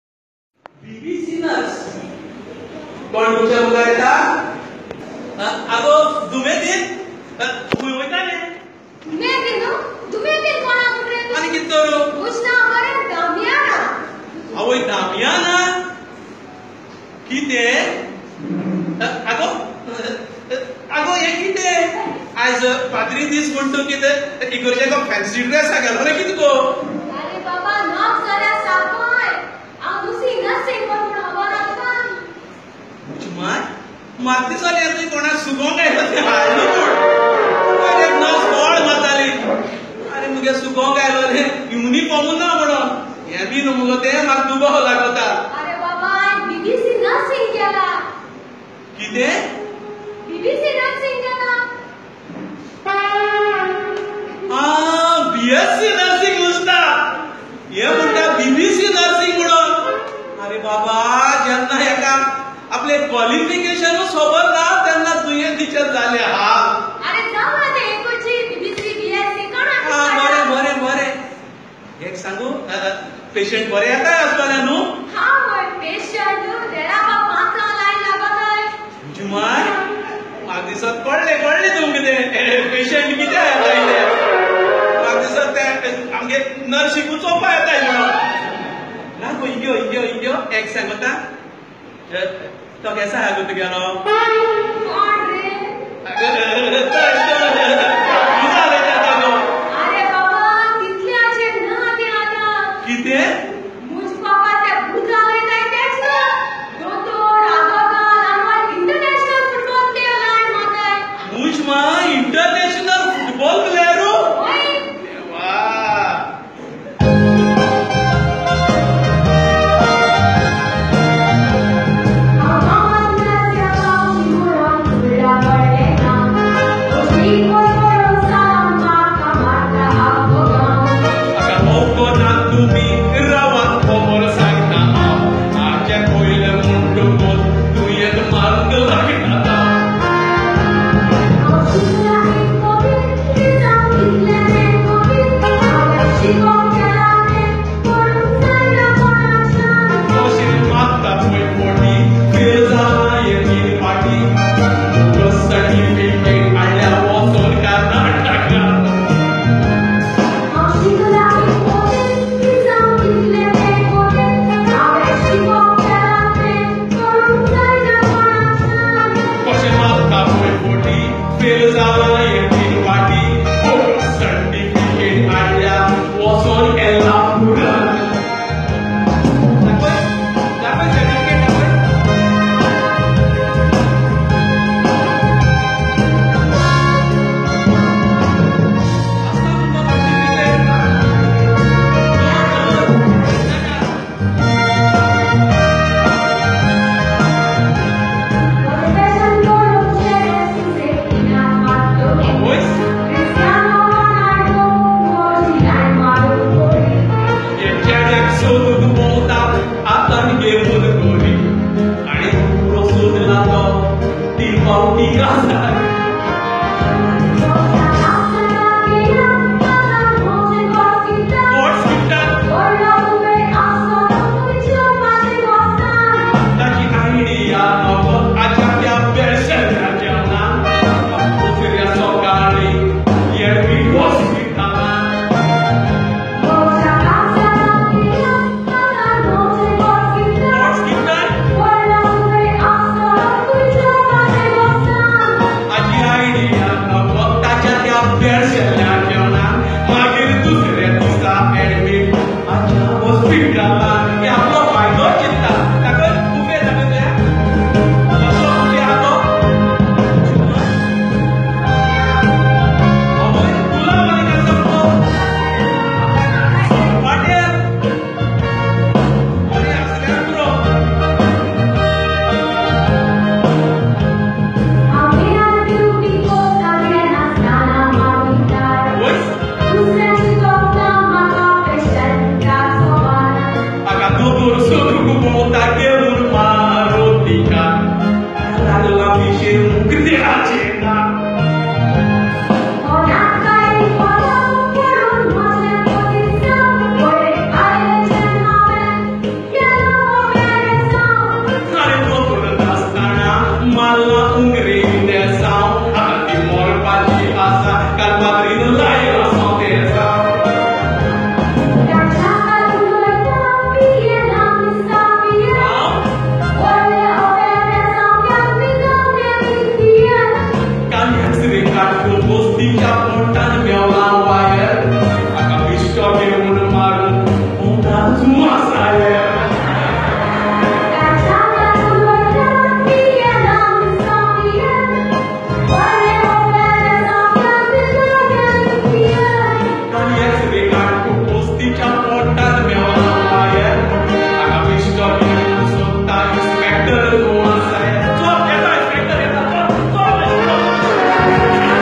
Mind blowing Konkani comedy song | Alleluia Pereira & Joycylin singing at Pastor's day celebration
Video from Peter Dias